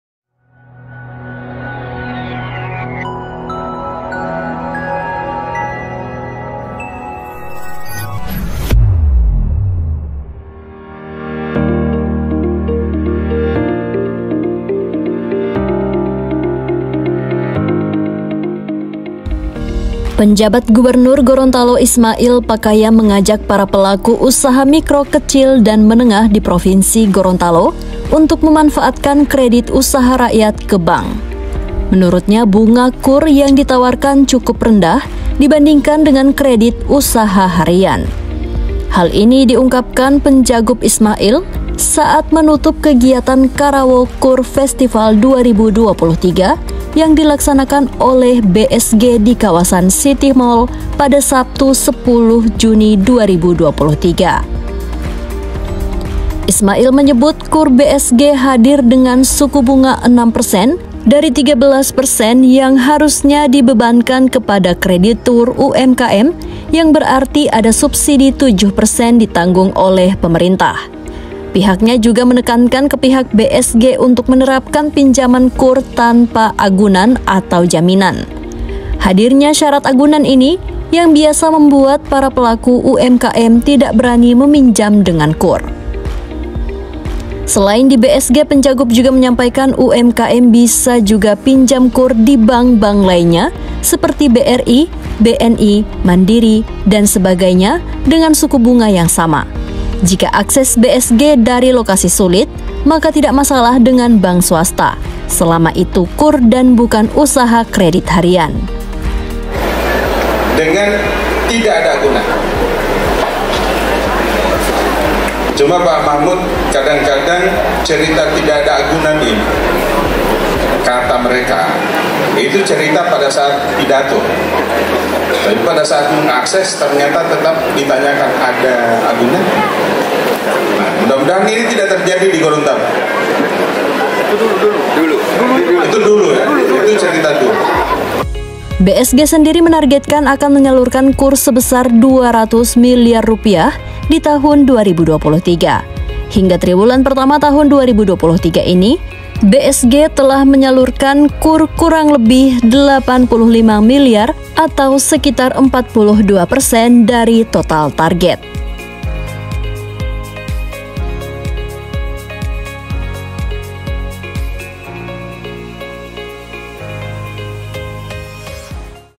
Penjagub Gorontalo Ajak Pelaku UMKM Pakai KUR
Kota Gorontalo, Kominfotik – Penjabat Gubernur Gorontalo Ismail Pakaya, mengajak para pelaku usaha mikro, kecil, dan menengah (UMKM) di Provinsi Gorontalo untuk memanfaatkan Kredit Usaha Rakyat (KUR) ke bank. Menurutnya bunga KUR yang ditawarkan cukup rendah, dibandingkan dengan kredit usaha harian.

“Saya kampanye KUR malam ini. Karena bunganya rendah, tidak ada angunan dan mudah di akses. Jika ada pengurus koperasi harian di sini, saya mohon maaf. Saya ini pernah jadi pembina koperasi dan itu lama, tapi saya menganjurkan UMKM kita di Gorontalo pinjam KUR saja,” ujar penjagub, saat menutup kegiatan Karawo KUR Festival 2023 yang dilaksanakan oleh BSG di kawasan City Mall, Sabtu, (10/3/2023).

Ismail menyebut KUR BSG hadir dengan suku bunga enam persen, dari 13 persen yang seharusnya dibebankan kepada kreditur UMKM, yang berarti ada subsidi tujuh persen ditanggung oleh pemerintah. Pihaknya juga menekankan ke pihak BSG untuk menerapkan pinjaman KUR tanpa agunan (jaminan). Hadirnya syarat agunan ini, yang biasa membuat para pelaku UMKM tidak berani meminjam dengan KUR.

“Tadi saya pun masih tau tetap ada agunan (jaminan), tapi di BSG katanya tidak ada agunan lagi. Jadi ibu, bapak kalau nanti masih ada agunan, datang lapor ke saya di rumah jabatan, nanti saya kejar pak Machmud dan pak Sahron,'” kata Ismail sedikit bercanda

Selain di BSG, Penjagub juga menyampaikan UMKM bisa juga pinjam KUR di bank – bank lainnya seperti BRI, BNI, Mandiri dan sebagainya, dengan suku bunga yang sama. Jika akses BSG dari lokasi sulit, maka tidak masalah dengan bank swasta, selama itu KUR dan bukan usaha kredit harian.

BSG sendiri menargetkan akan menyalurkan KUR sebesar Rp200 miliar di tahun 2023. Hingga triwulan pertama tahun 2023 ini, BSG telah menyalurkan KUR kurang lebih Rp85 miliar atau sekitar 42 persen dari total target.

Pewarta : Echin